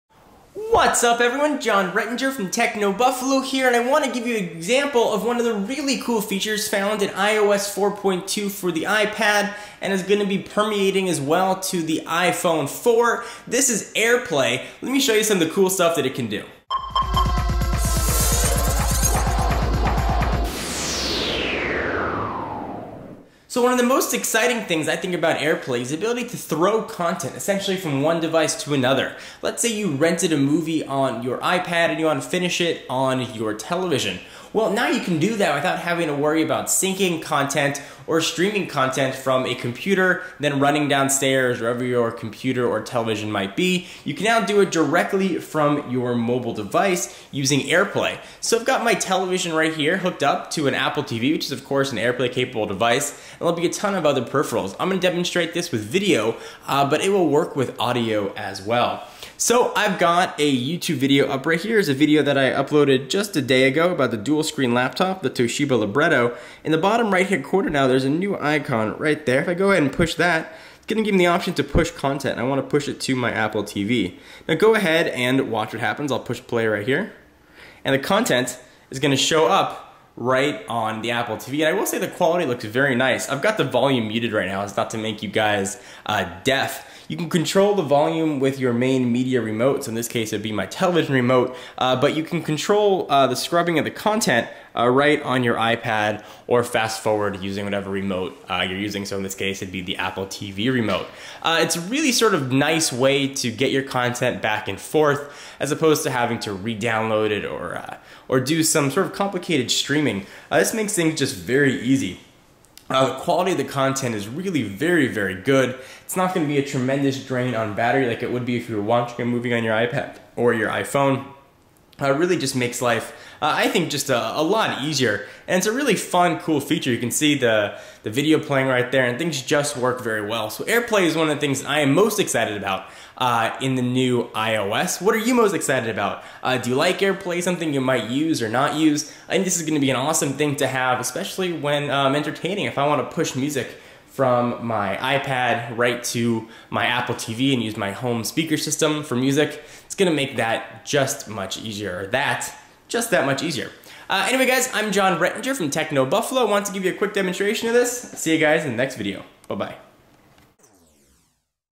AirPlay: New iOS Feature
AirPlay, the new technology built into iOS that lets you 'throw' content from one device to the next.  Aside from multitasking, this is the feature I've been most looking forward to.  You?

For more:
TechnoBuffalo:

iOS 4.2 AirPlay